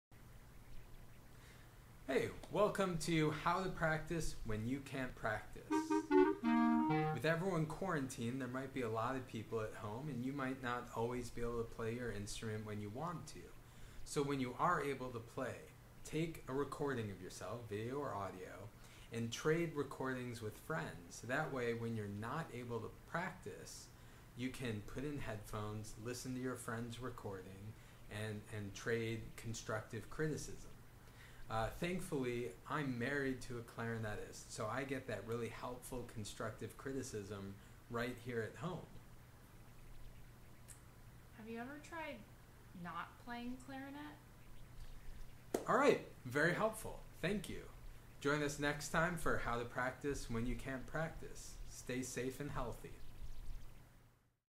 Trade Clarinet Videos with a Friend
Everyone is home and no one wants to hear you practice? Here's a helpful tip for how to get around this road block.